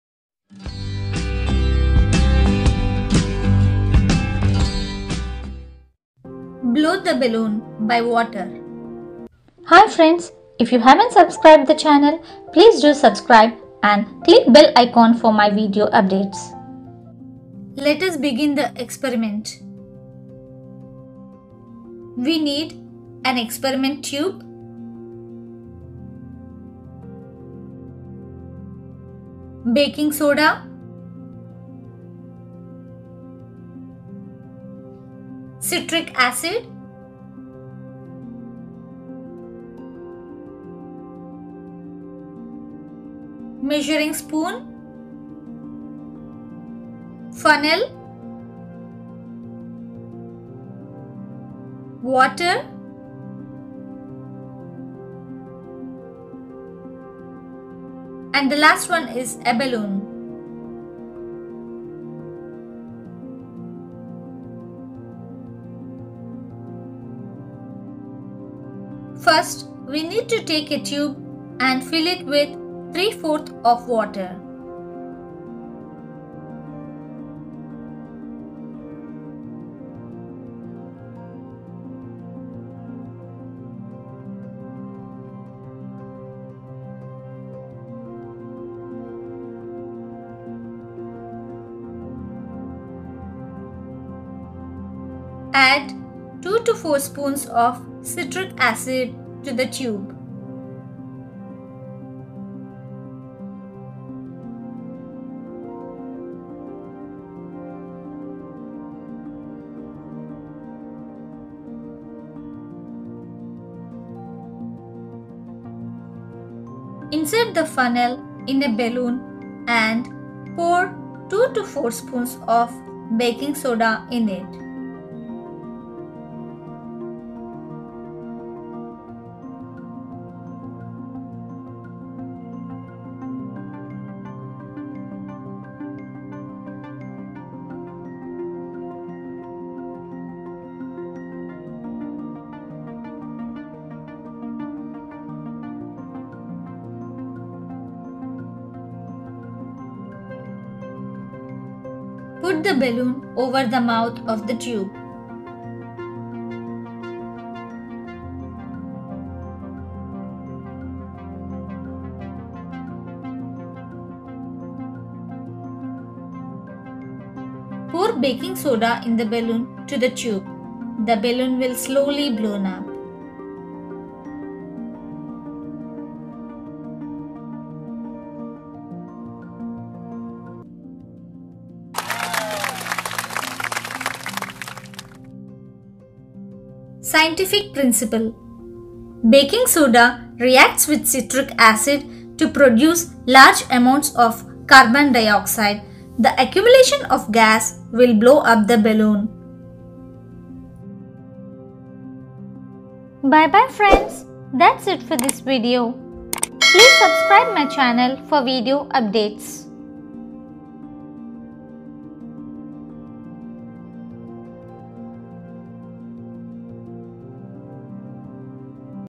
Balloon Experiment || Blowing a Balloon with baking soda and Citric Acid
This experiment shows the reaction between citric acid and baking soda with water. It helps balloon to float. You need to watch the complete video to understand the reason behind it.

This channel is created for all ages for entertainment, fun and learning. Please provide comments/suggestions.